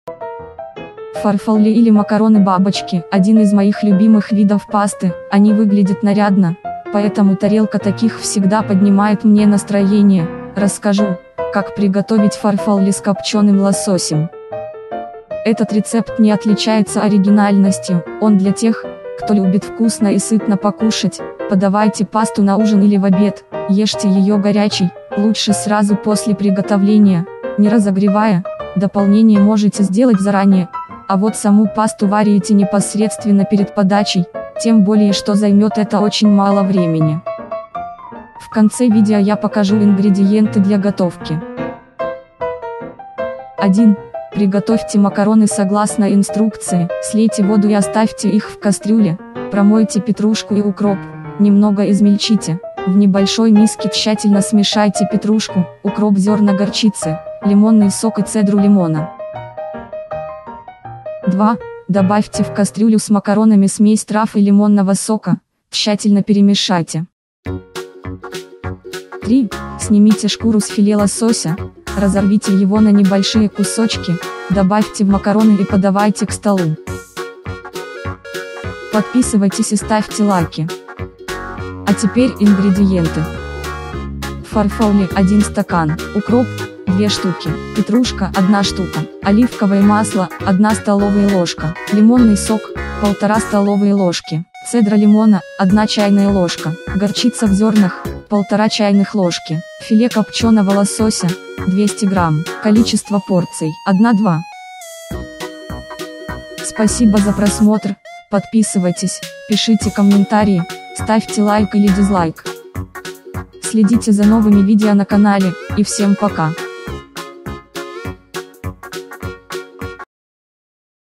★ДОМАШНЯЯ ЕДА!★ ФАРФАЛЛЕ С КОПЧЕНЫМ ЛОСОСЕМ ★
★Ошеломляющий вкус! Фарфалле или макароны-бабочки – один из моих любимых видов пасты● Они выглядят нарядно, поэтому тарелка таких всегда поднимает мне настроение● Расскажу, как приготовить фарфалле с копченым лососем●